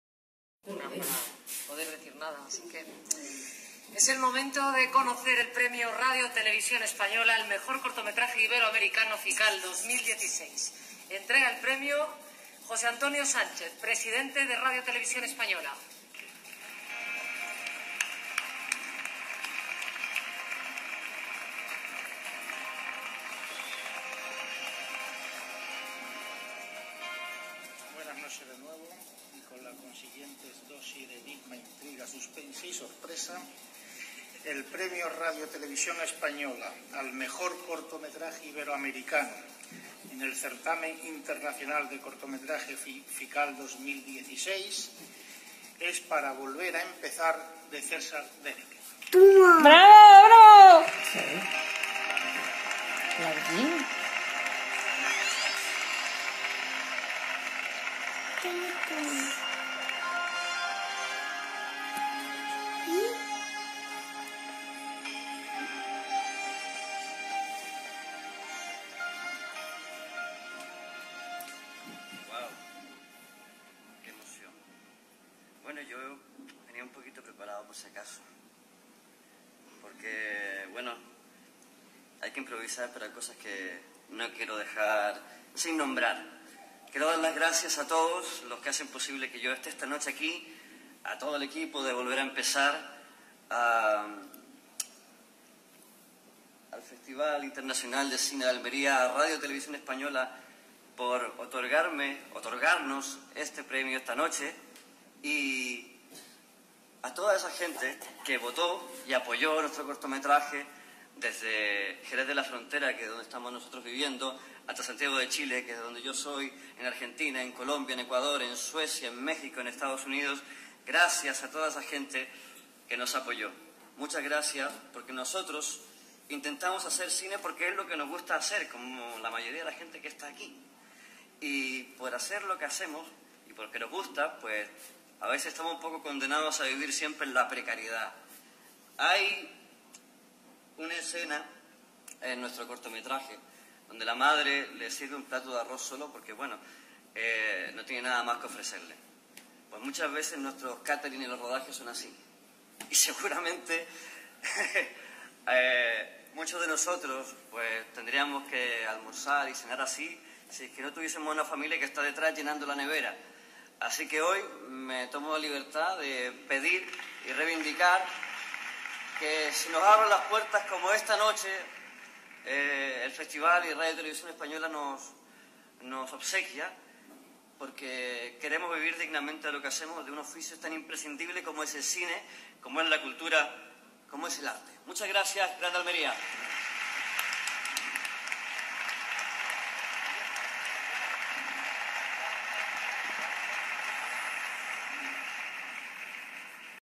Premio al mejor cortometraje iberoamericano por Volver a empezar.
Premio al mejor cortometraje iberoamericano por Volver a empezar. Festival Internacional de cine de Almería 2016